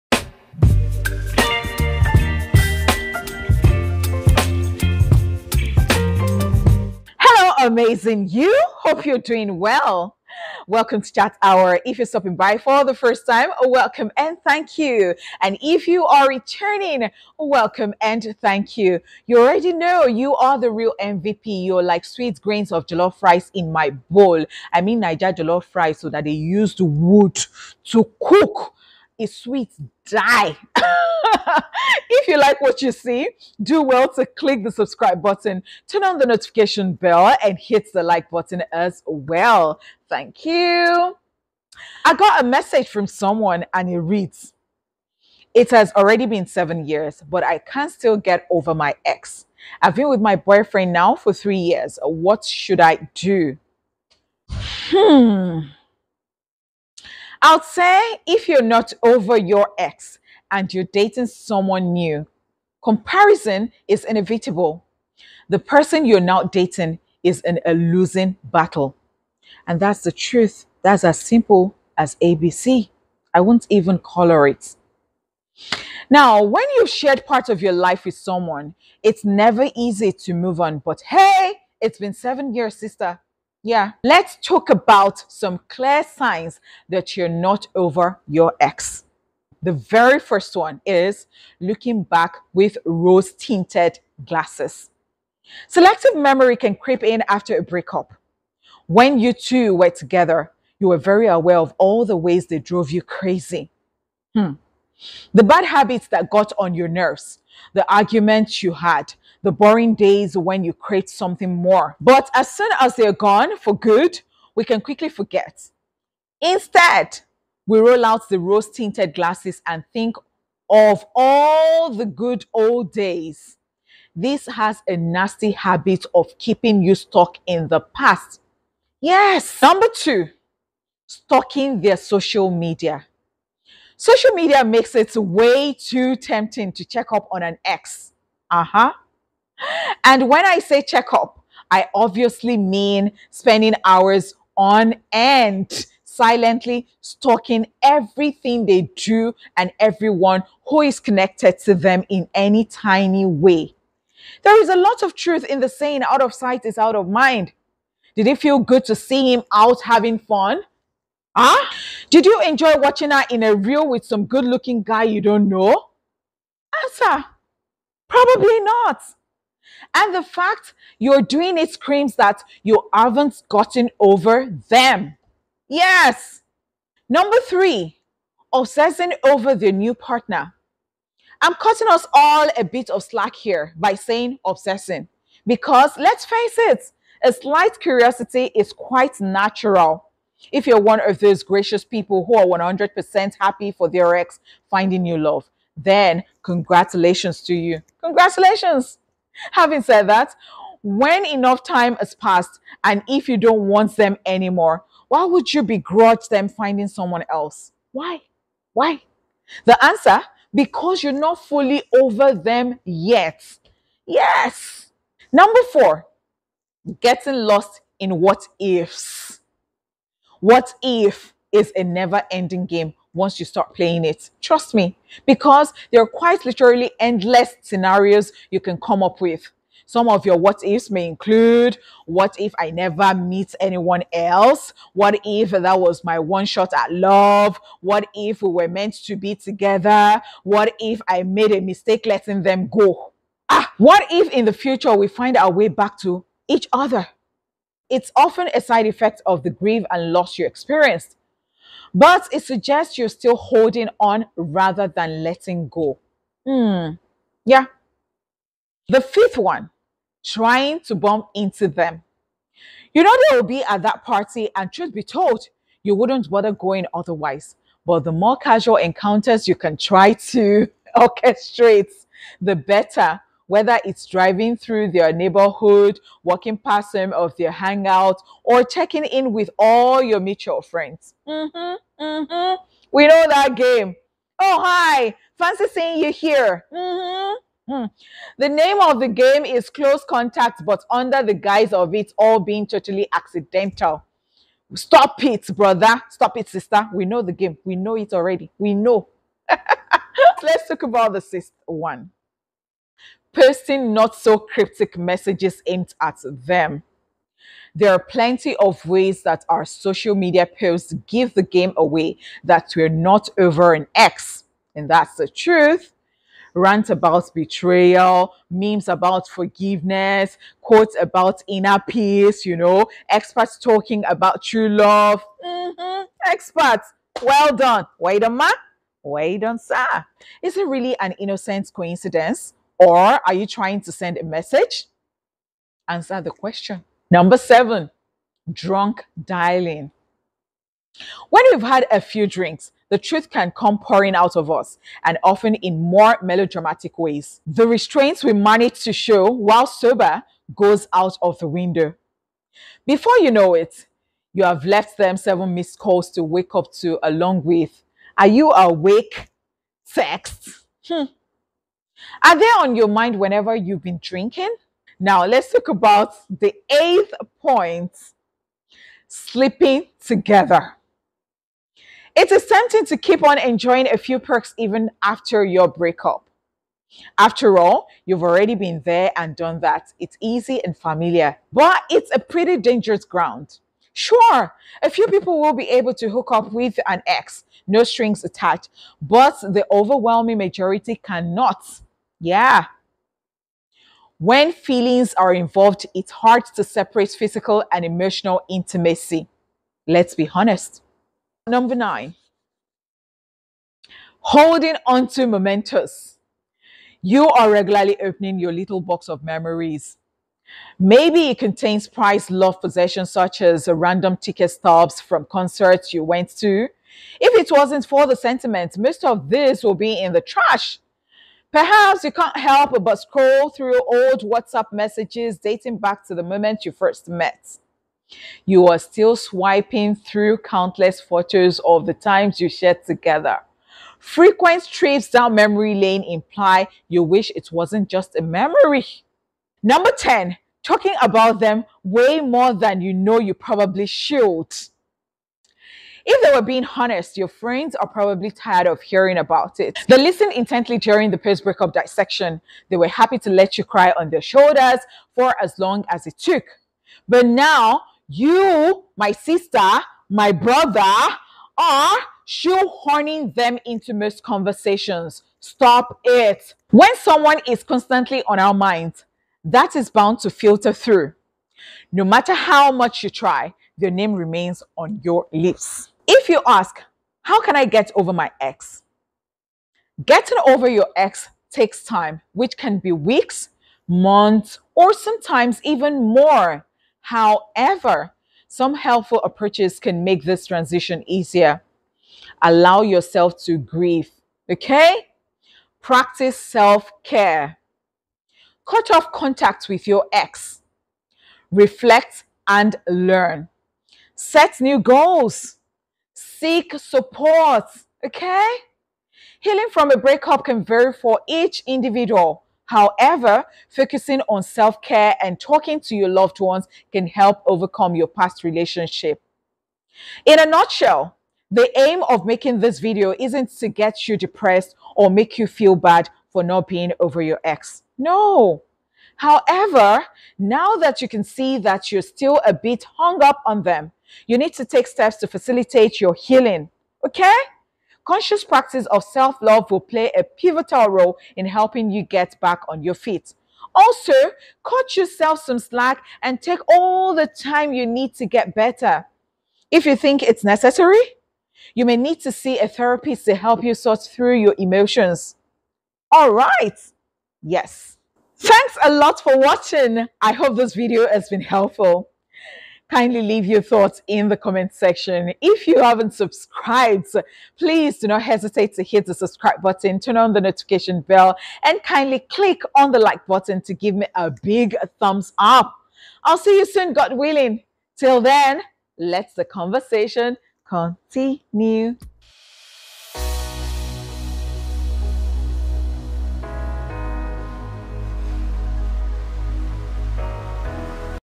It's Been 7 Years, Why Can't I Get Over My Ex?
So, you may have walked away from the relationship and decided you can do better for yourself than remain with your ex. And you might be feeling good because you’ve got your life back on track (or, at least, you are close to achieving that).

However, it may have been weeks (or even years) since the breakup, but something might not feel right as you can’t seem to get them out of your mind.